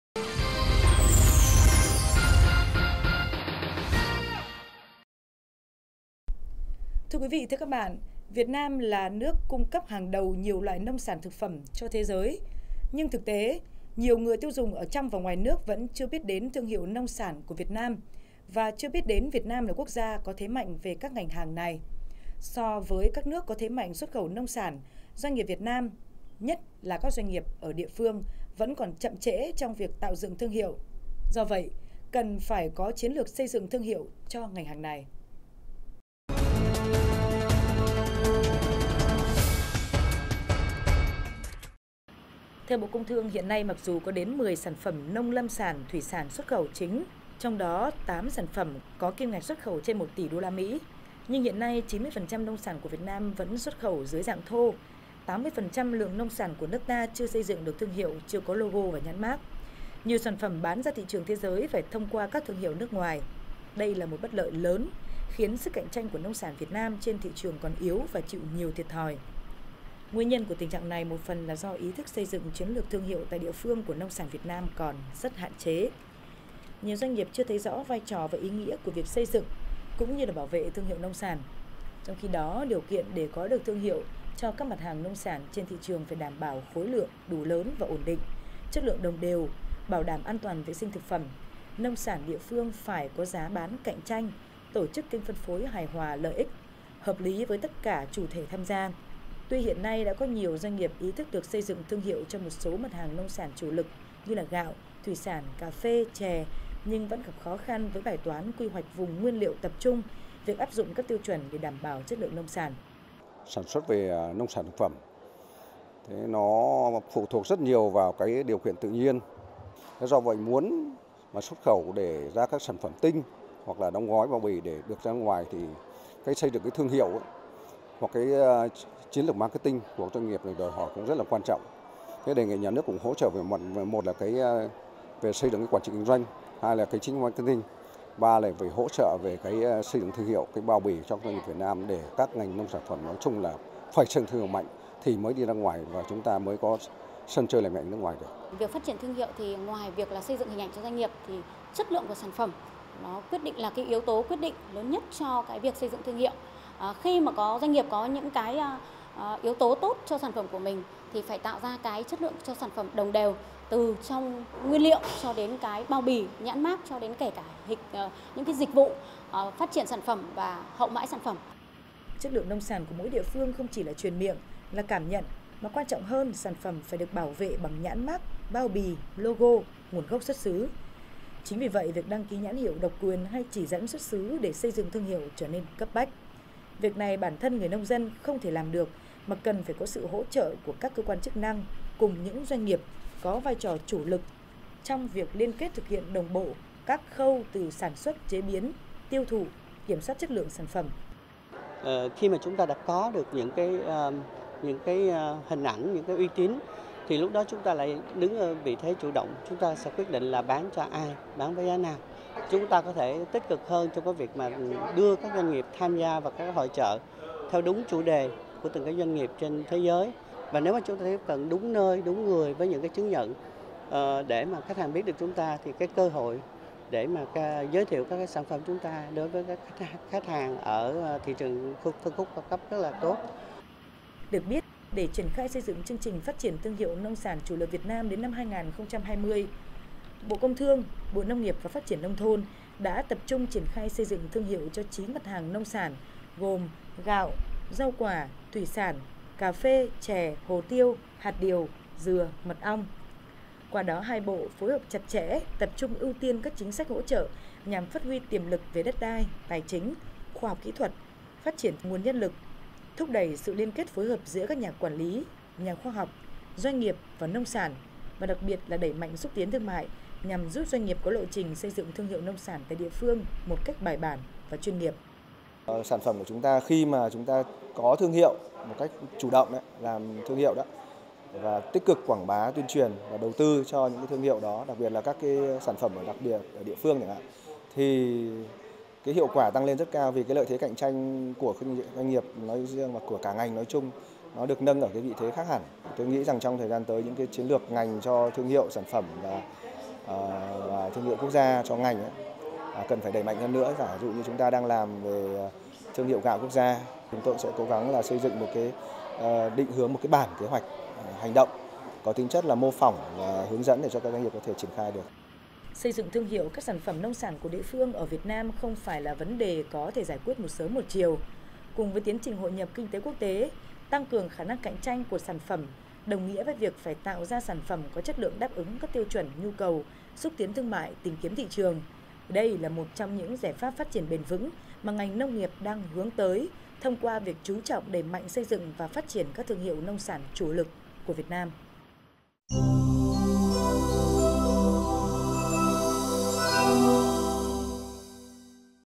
Chiến lược xây dựng thương hiệu cho sản phẩm nông sản địa phương
Việt Nam là nước cung cấp hàng đầu nhiều loại nông sản, thực phẩm cho thế giới nhưng thực tế nhiều người tiêu dùng trong và ngoài nước vẫn chưa biết đến các thương hiệu nông sản của Việt Nam, chưa biết đến Việt Nam là quốc gia có thế mạnh về các ngành hàng này. So với nhiều nước có thế mạnh xuất khẩu nông sản, doanh nghiệp Việt Nam nhất là các doanh nghiệp ở địa phương vẫn còn chậm trễ trong việc tạo dựng thương hiệu. Do vậy, cần phải có chiến lược xây dựng thương hiệu cho ngành hàng này...